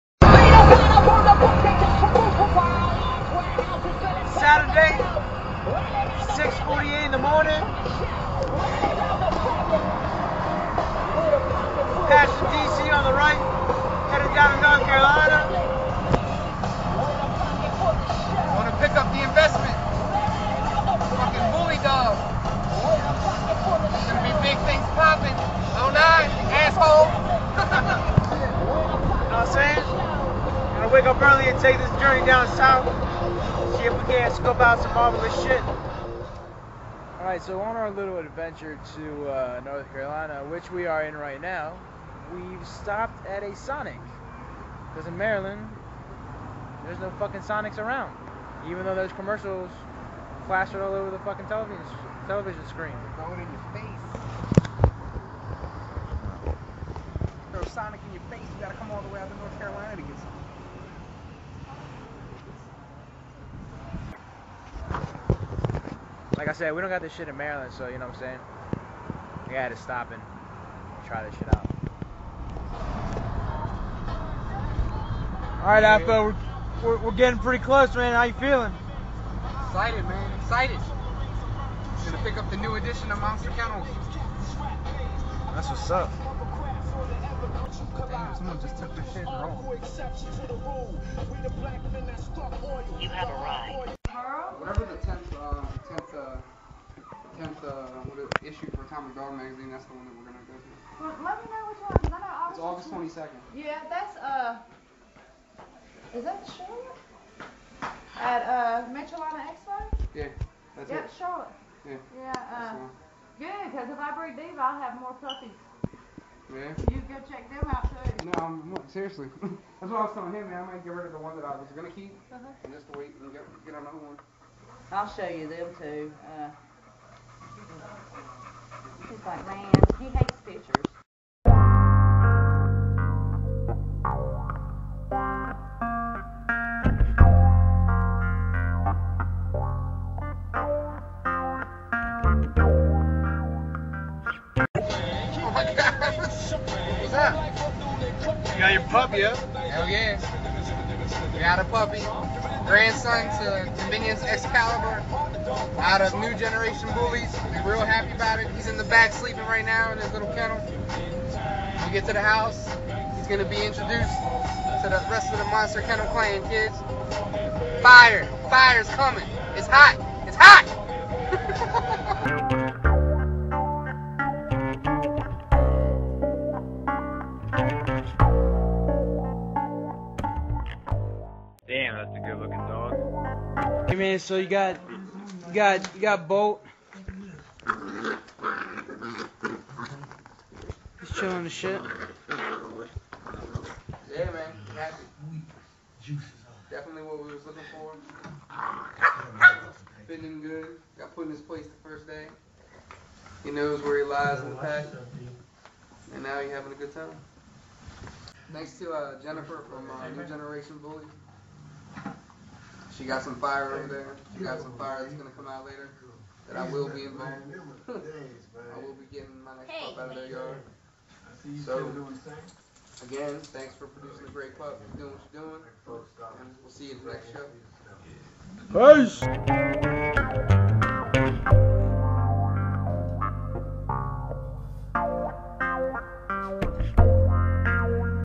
Another Puppy added to Monter Kennels
Trip to North Carolina to pick up a pup